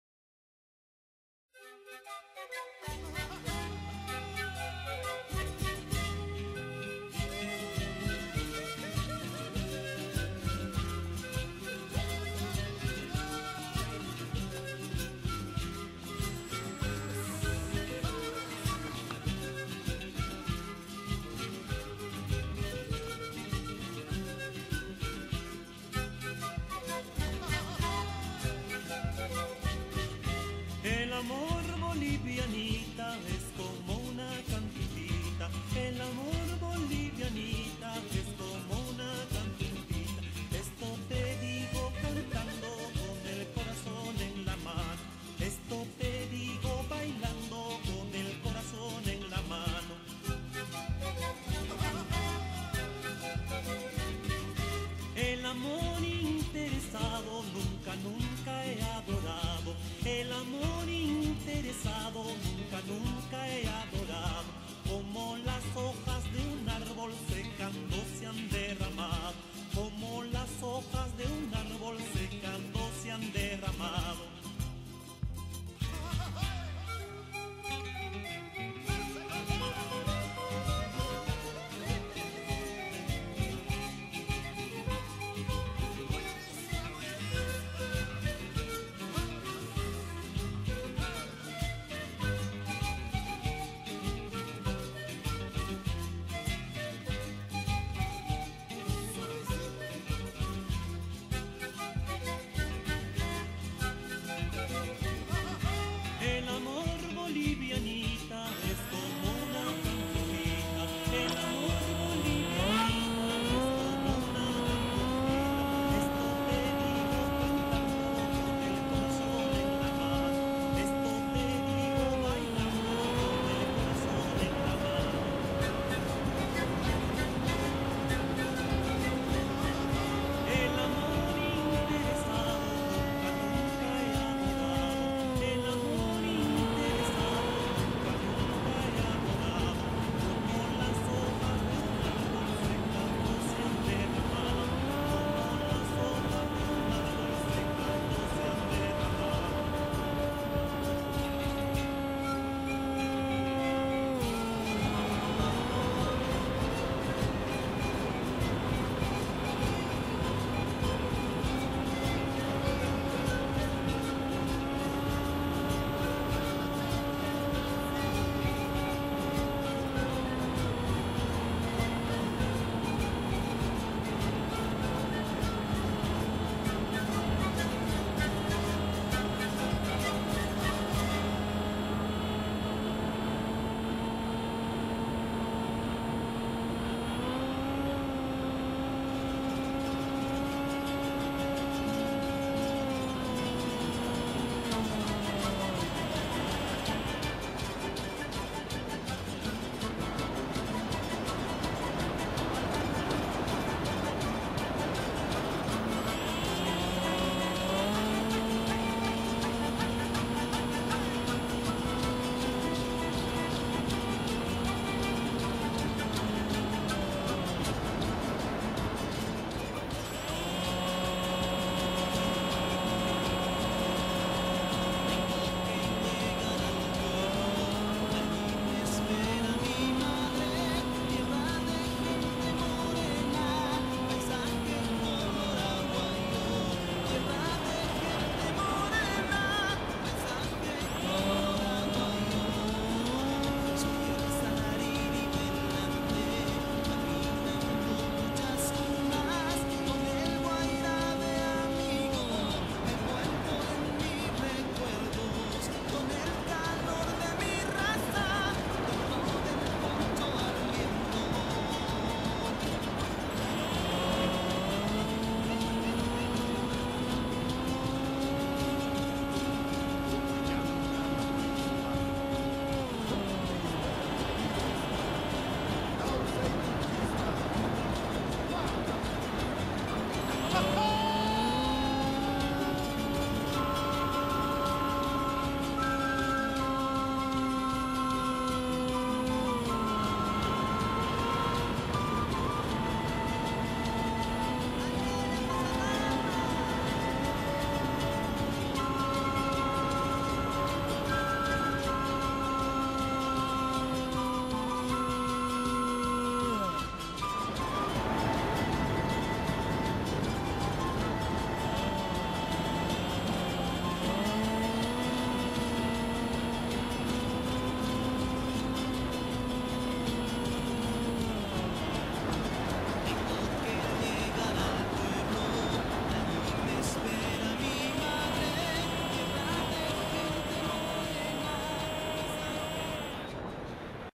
Eachine Micro SkyHunter with Onepaa X2000 (2018/5/10)
Eachine Micro SkyHunter に
Onepaa X2000 2Kアクションカメラを搭載して
空撮してみました。